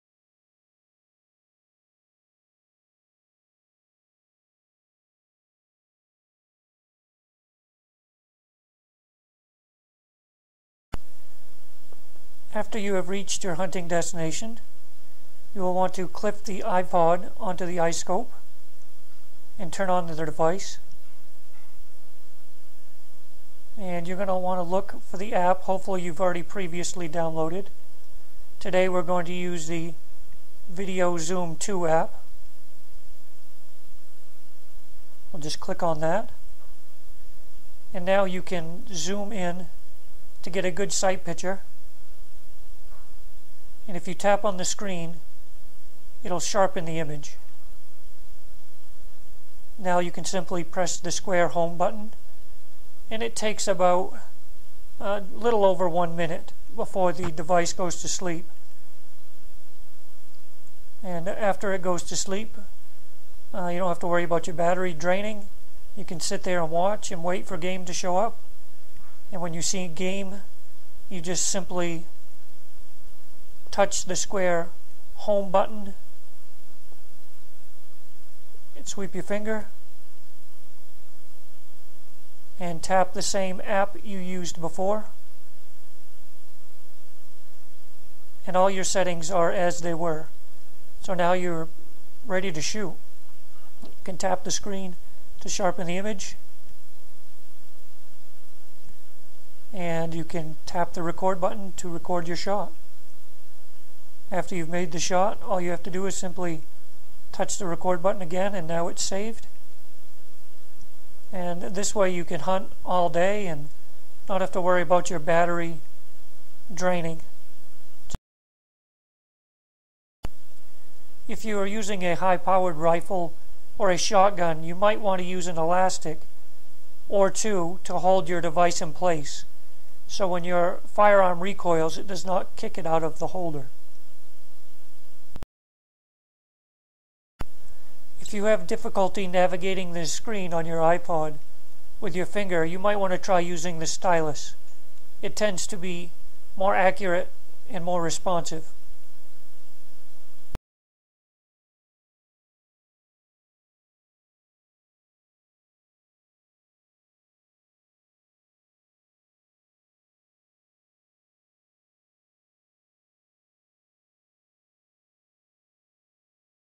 Iscope and ipod setup
Video showing how to setup and conserve battery power when using the Iscope with an ipod touch 5th generation.